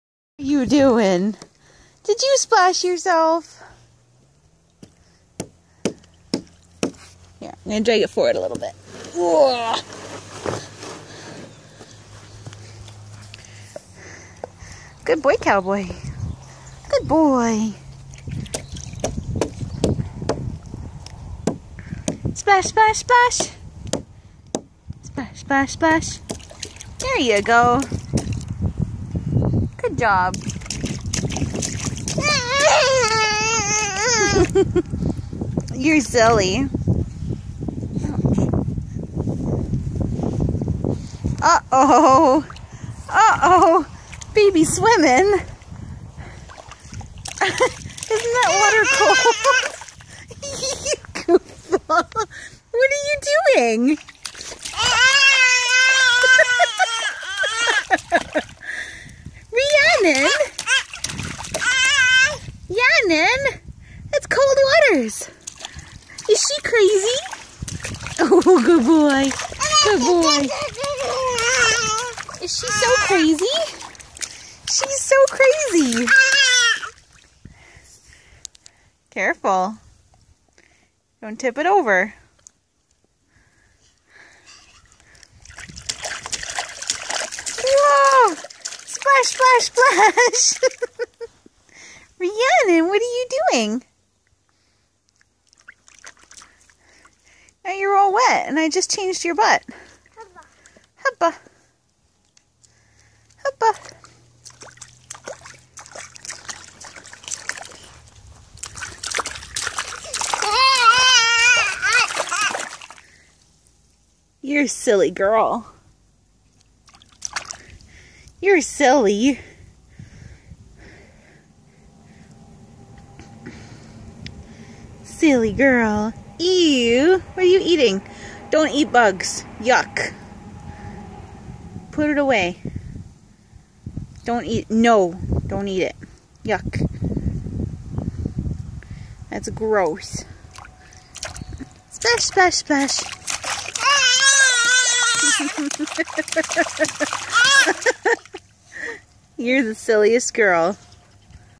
Baby In The Bird Bath
Some kids hate bath time.  My kids actively seeks out bath opportunities.  Even if it's cold.